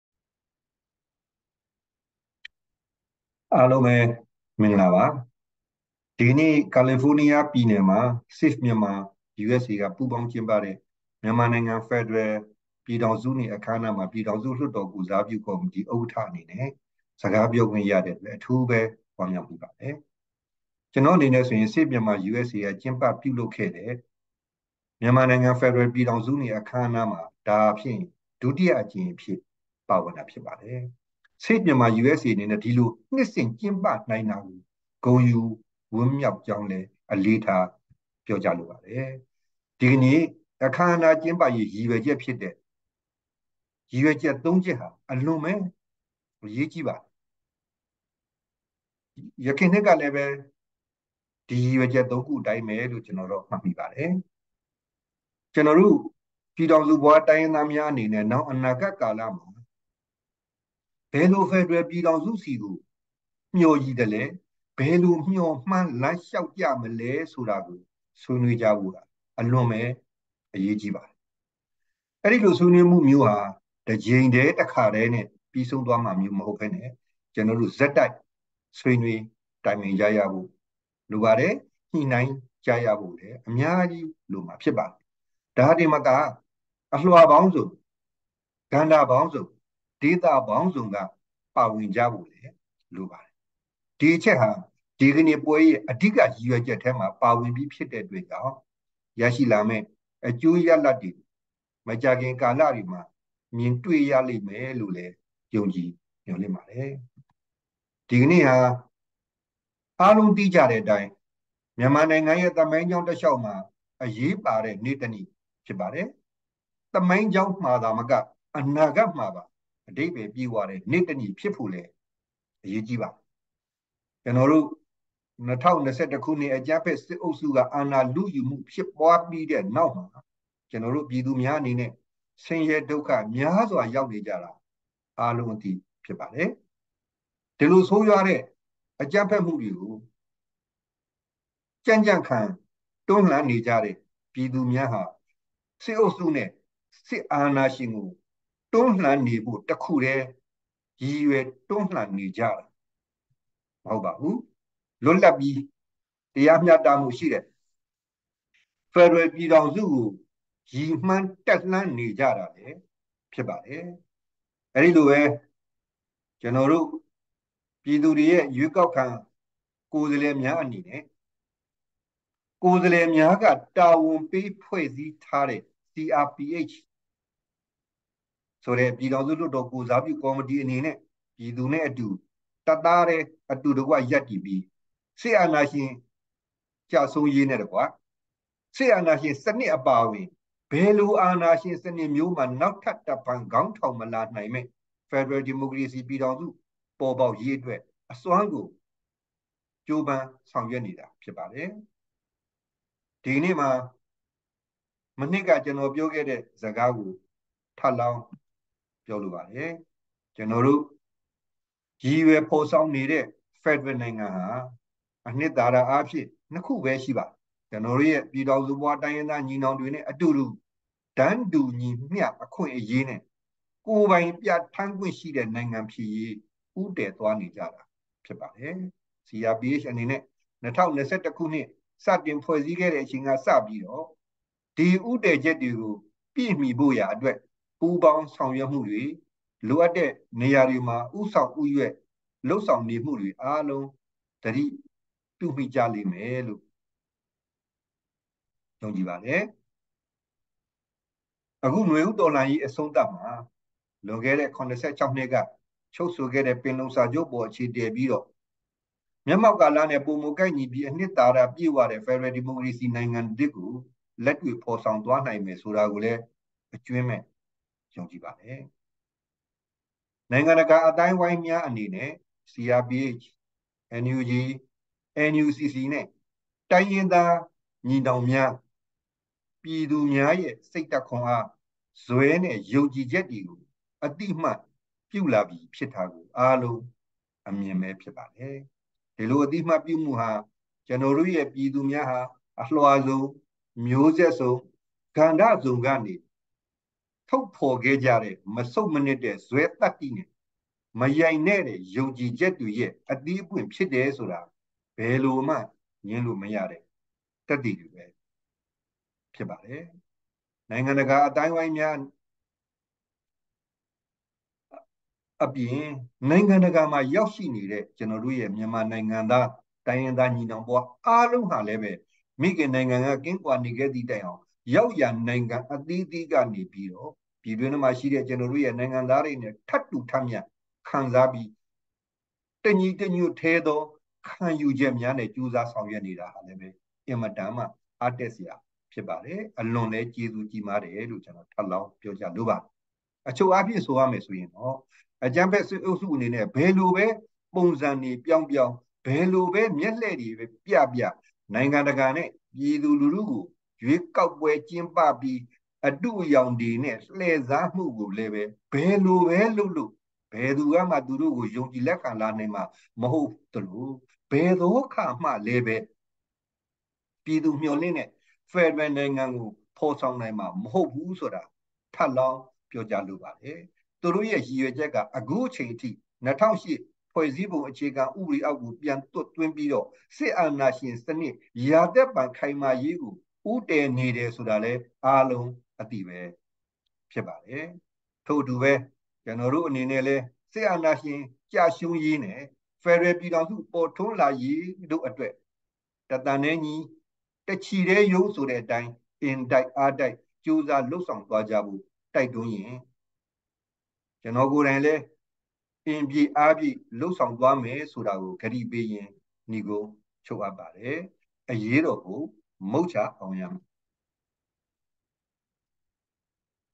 CRPH ဥက္ကဋ္ဌ ဦးအောင်ကြည်ညွန့်၏ “မြန်မာနိုင်ငံဖက်ဒရယ်ပြည်ထောင်စုနေ့” အထိမ်းအမှတ် မိန့်ခွန်း
၁၁-၂-၂၀၂၃ ရက်နေ့၊ Save Myanmar–USA မှ ဦးစီးကျင်းပခဲ့သော “မြန်မာနိုင်ငံ ဖက်ဒရယ် ပြည်ထောင်စုနေ့” အခမ်းအနားတွင် ပြောကြားခဲ့သည့် ပြည်ထောင်စုလွှတ်တော်ကိုယ်စားပြုကော်မတီ ဥက္ကဋ္ဌ ဦးအောင်ကြည်ညွန့်၏ မိန့်ခွန်း